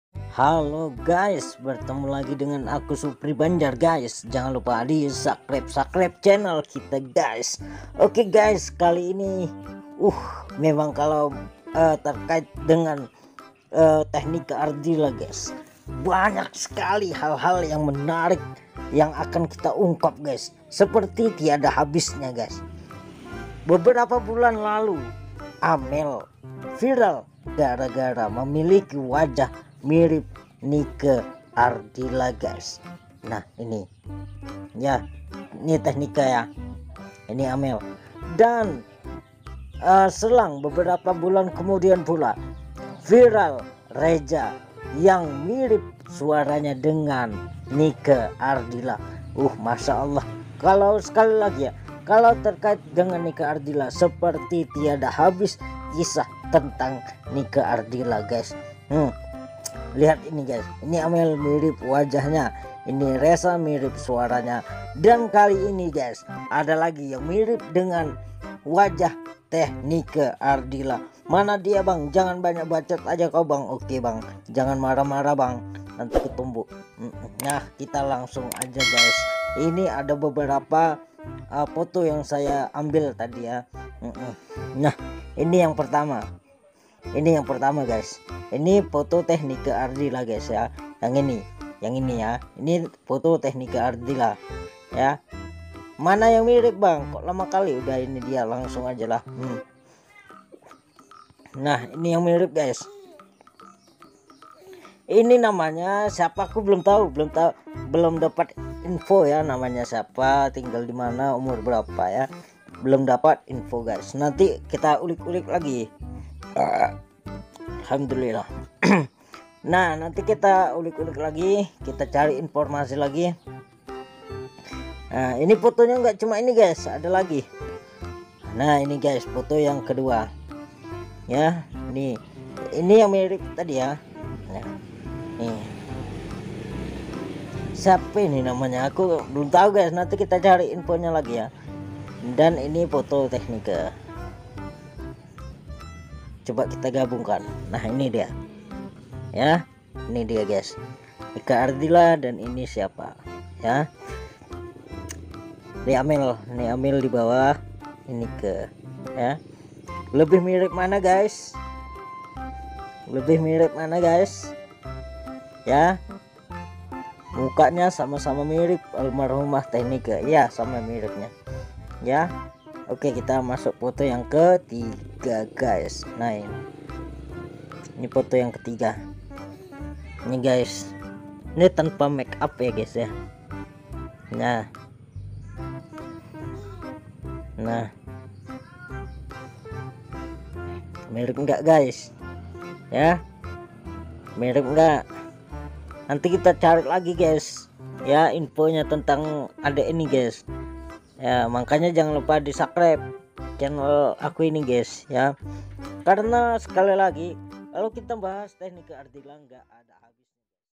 viral, gadis ini juga mirip Nike Ardilla.! keberkahan Nike Ardilla luar biasa.
dalam video kali ini saya akan memperlihatkan seorang gadis yang belum diketahui keberadaan dan identitasnya yang sangat mempunyai kemiripan wajah dengan Nike Ardilla Tak ubahnya dengan Amel yang sangat mirip dengan Nike Ardila kini bermunculan beberapa orang yang mirip dengan Nike Ardilla dan simak video kali ini dan marilah kita dukung semua yang berkaitan dengan Nike Ardilla...